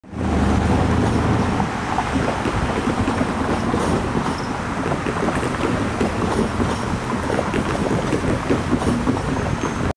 Amtrak Superliner going through Downers Grove very slowly
A westbound Amtrak at 2:28:28 on June 29, 2017. I made this video to record how slow it was going. The track speed limit for passenger trains in Downers is 70mph. They normally "fly" through town.